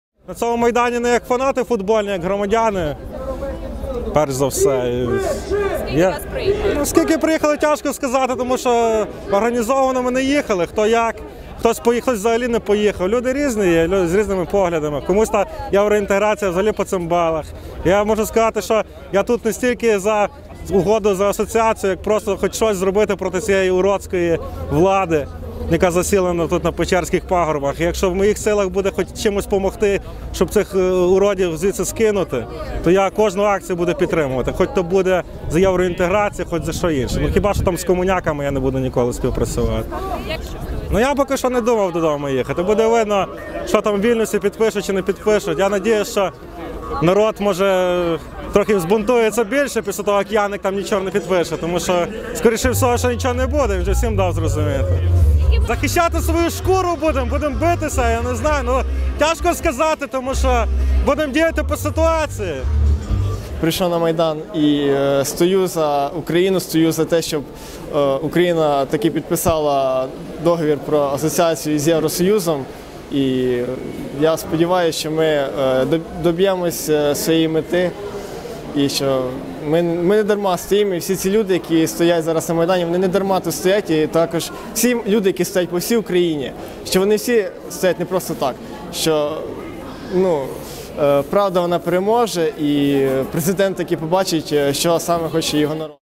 Football Fans at Maidan: Unity in the Struggle (2013–2014)
Football fans played a significant role during the Revolution of Dignity. At Maidan, they united to support the protesters and defend the ideals of freedom and justice. This video captures their presence, chants, and active participation in the confrontation, symbolizing the unity of the people. Archival footage conveys the unique atmosphere of the struggle, where even football culture became a part of the revolution.